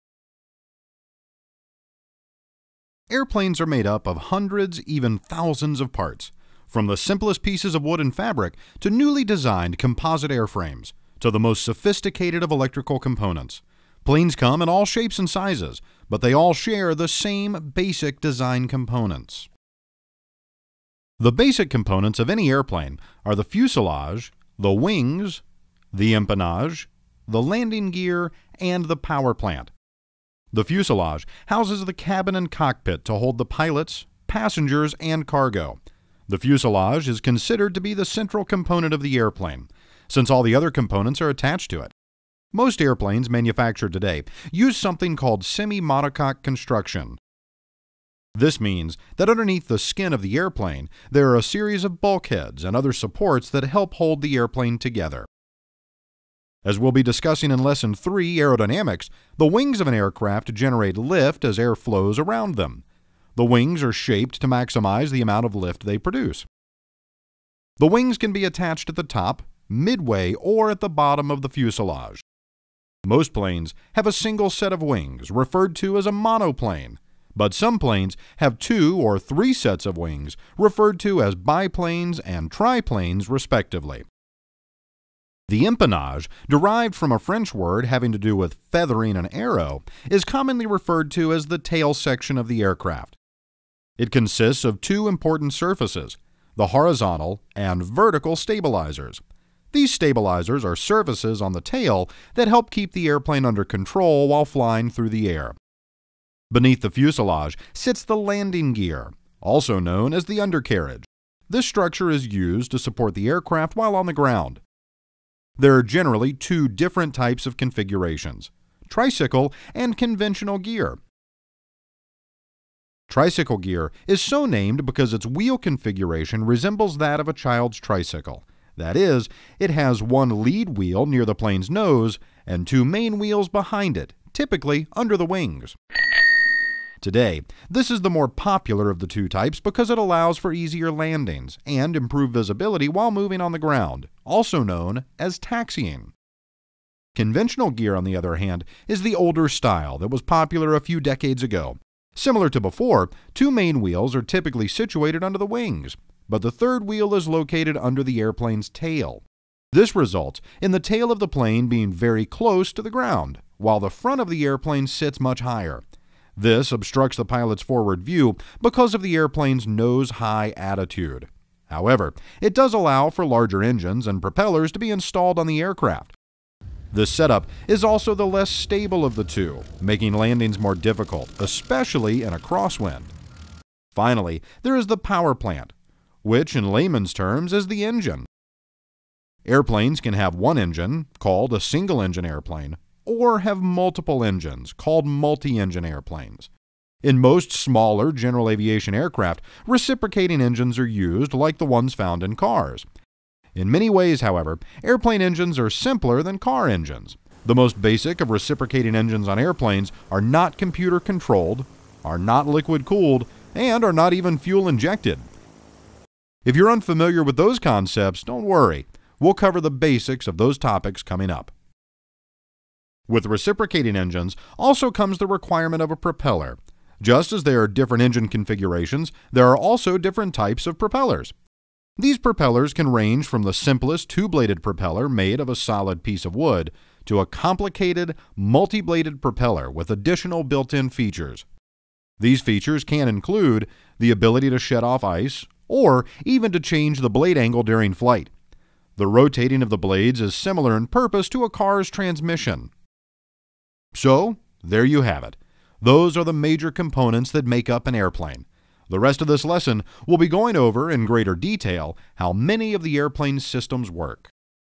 Aircraft Systems - 01 - Parts of an Airplane
Here you will learn all of the major components that are common to most types of airplanes.